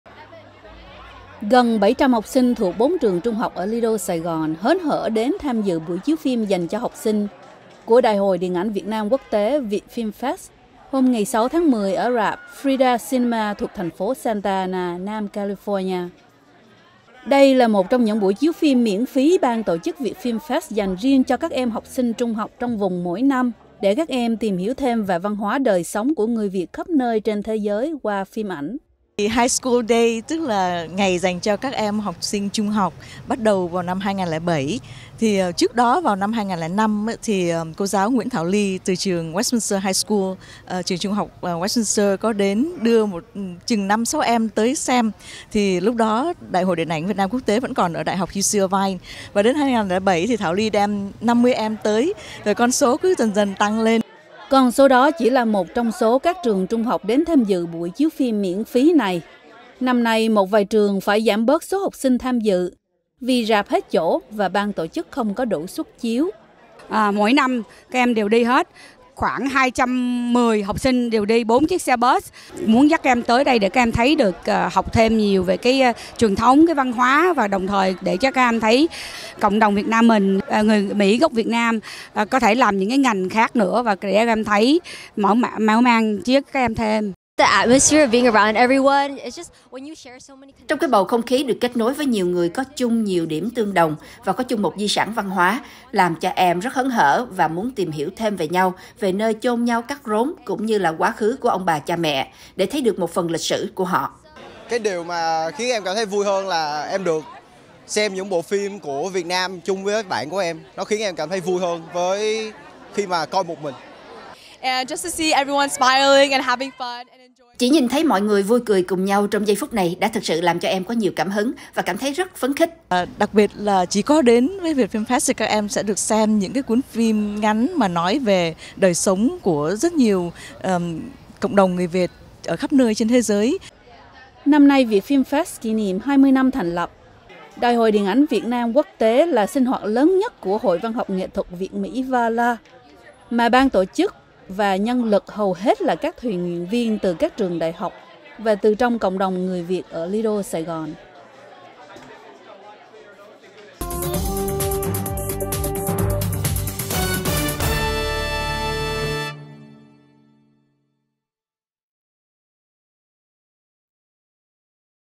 Học sinh ở Little Saigon hớn hở đi xem phim Việt | VOA Tiếng Việt
Gần 700 học sinh thuộc 4 trường trung học ở Little Saigon hớn hở đến tham dự buổi chiếu phim dành cho học sinh của Đại Hội Điện Ảnh Việt Nam Quốc Tế VietFilmFest hôm 6/10 ở rạp Frida Cinema thuộc thành phố Santa Ana, Nam California.

Đây là một trong những buổi chiếu phim miễn phí ban tổ chức VietFilmFest dành riêng cho các em học sinh trung học trong vùng mỗi năm, để các em tìm hiểu thêm về văn hoá đời sống của người Việt khắp nơi trên thế giới qua phim ảnh.

YSA LÊ, Đồng sáng lập VietFimFest/Giám đốc điều hành hội VAALA

High School Day tức là ngày dành cho các em học sinh trung học bắt đầu vào năm 2007 thì trước đó vào năm 2005 thì cô giáo Nguyễn Thảo Ly từ trường Westminster High school, trường trung học Westminster có đến đưa khoảng chừng 5,6 em tới xem thì lúc đó Đại Hội Điện Ảnh Việt Nam Quốc Tế vẫn còn ở UC Irvine. Và đến 2007 thì Thảo Ly em 50 em tới và con số cứ dần tăng lên.

Con số đó chỉ là một trong số các trường trung học đến tham dự buổi chiếu phim miễn phí này. Năm nay một vài trường phải giảm bớt số học sinh tham dự vì rạp hết chỗ và ban tổ chức không có đủ xuất chiếu.

VI ĐÀO, Giáo viên Việt Ngữ Trường Trung học La Quinta, Học khu Garden Grove
Mỗi năm các em đều đi hết, khoảng 210 học sinh đi bốn chiếc xe bus, muốn dắt các em tới đây để các em thấy được, học thêm nhiều về cái truyền thống cái văn hoá đồng thời để cho các em thấy cộng đồng Việt Nam mình, người Mỹ gốc Việt Nam có thể làm những cái ngành khác nữa và để cho các em thấy và mở mang trí ức của các em thêm.

ANGELINA TRẦN, Lớp 11 Trường Trung học La Quinta
Trong cái bầu không khí được kết nối với nhiều người có chung nhiều điểm tương đồng, và có chung một di sản văn hoá, nó làm cho em rất hớn hở và muốn tìm hiểu thêm về nhau, về nơi chôn nhau cắt rốn, như là quá khứ của ông bà cha mẹ, để thấy được một phần lịch sử của họ.

TRẦN QUỐC THỊNH, Lớp 10 Trường La Quinta
Cái điều mà khiến em cảm thấy vui hơn là em được xem những bộ phim của Việt Nam chung với các bạn của em, nó khiến em cảm thấy vui hơn là khi coi một mình.

TÂM NGUYỄN, Lớp 12 Trường Trung học La Quinta
Chỉ nhìn thấy mọi người vui cười cùng nhau trong giây phút này đã thật sự làm cho em có nhiều cảm hứng và cảm thấy rất phấn khích.

Đặc biệt là chỉ khi đến với Việt Film Fest các em mới có dịp được xem những cuốn phim ngắn nói về đời sống của rất nhiều cộng đồng người Việt ở khắp nơi trên thế giới.

Năm nay ViệtFilmFest kỷ niệm 20 năm thành lập. Đại hội điện ảnh Việt Nam Quốc Tế là sinh hoạt lớn nhất của hội Văn Học Nghệ Thuật Việt Mỹ VAALA, mà ban tổ chức và nhân lực hầu hết là các thiện nguyện viên từ các trường đại học và từ trong cộng đồng người Việt ở Little Saigon.---